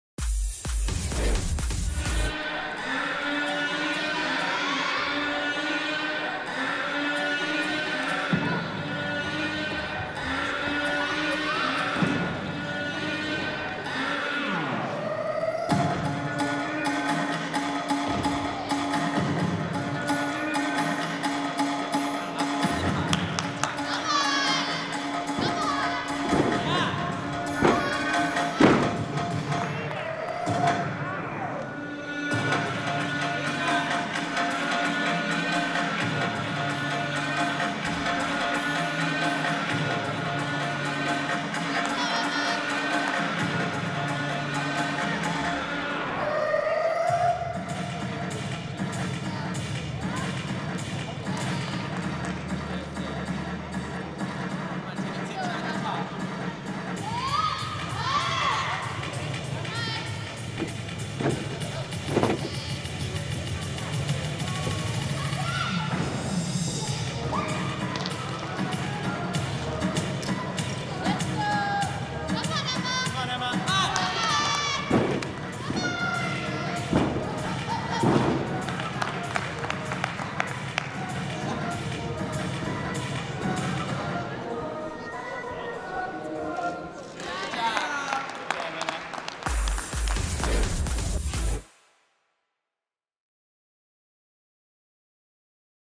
Emma Spence (CAN) - Floor Exercise - 2022 World Championships - Podium Training
Emma Spence on floor exercise during podium training at the 2022 World Championships in Liverpool, England.